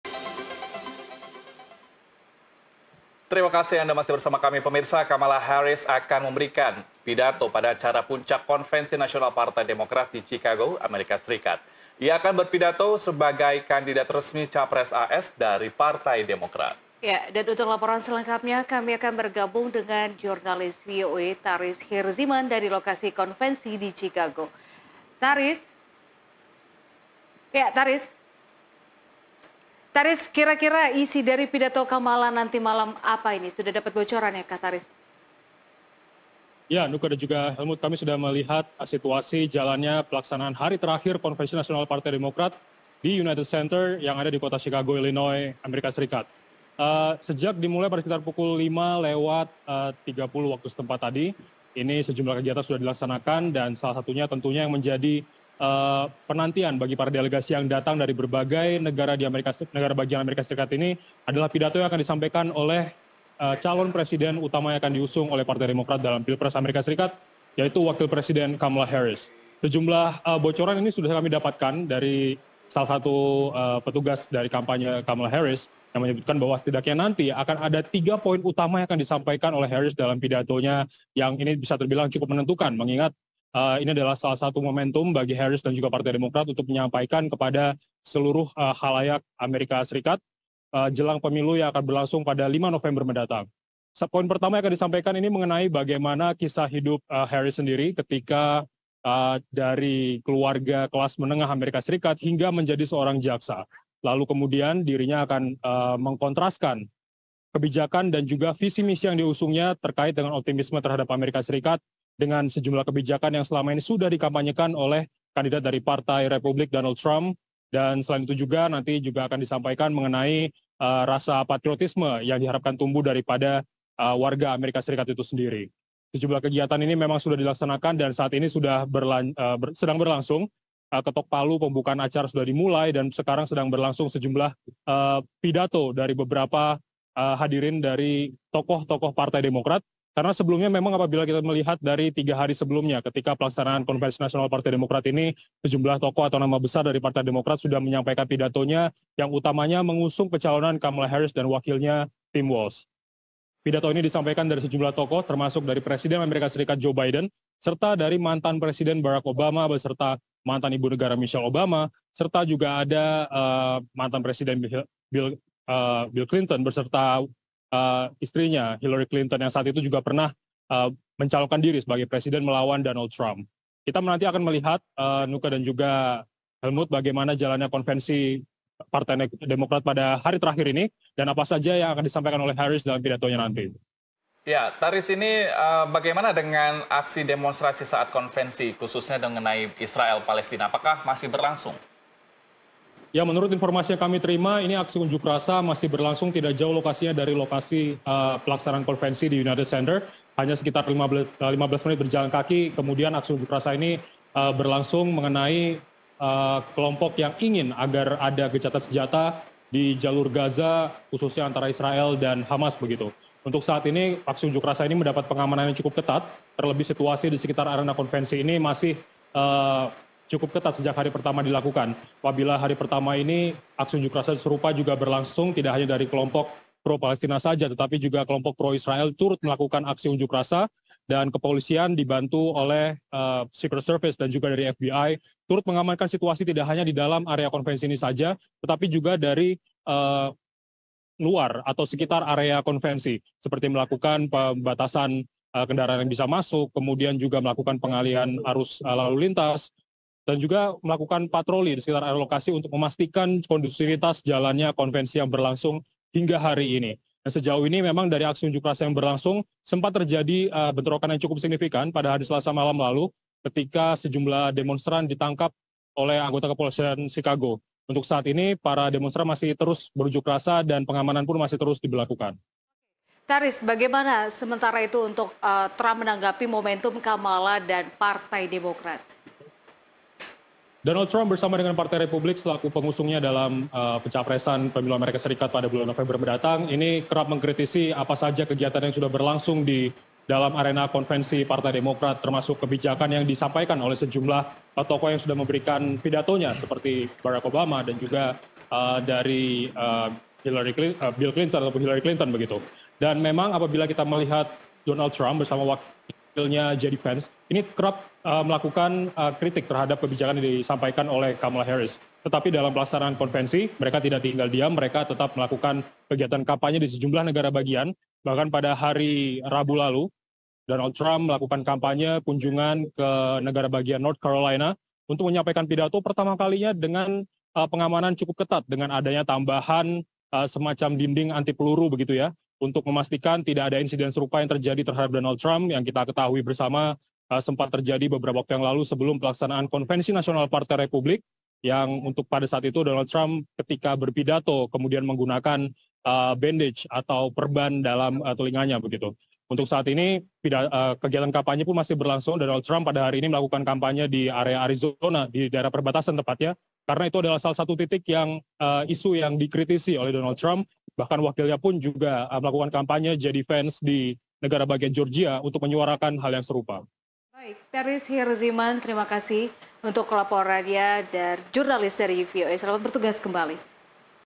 Laporan VOA untuk NTV: Kamala Harris Siap Terima Nominasi Capres Partai Demokrat
Reporter VOA, Taris Hirziman melaporkan langsung dari lokasi konvensi di Chicago untuk pemirsa Nusantara TV mengenai perkembangan terbaru dari Konvensi Nasional Partai Demokrat di mana Wapres Kamala Haris akan memberikan pidato dan menerima nominasi resmi sebagai Capres, Jumat pagi 23 Agustus 2024.